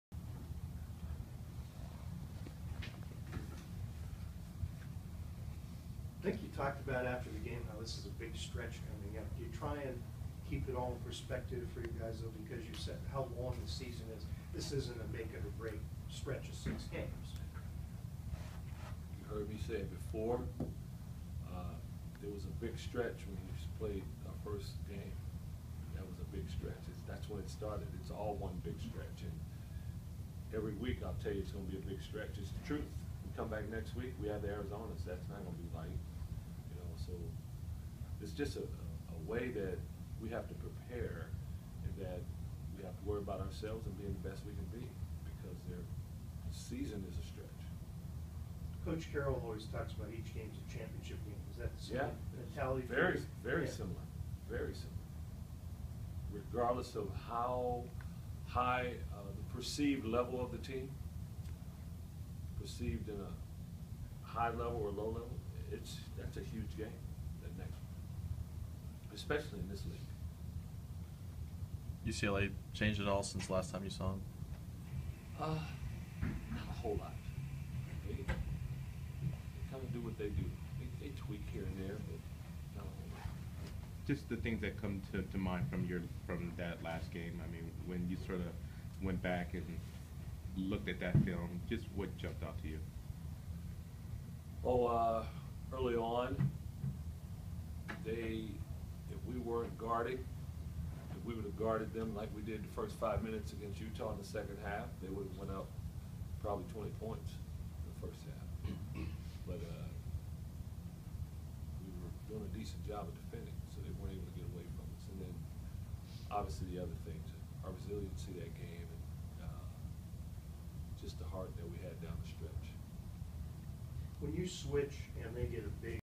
Lorenzo Romar previews UCLA, USC trip (1/26/16)
Washington Huskies men's basketball head coach Lorenzo Romar looks ahead to a California roadtrip, with visits at UCLA and USC.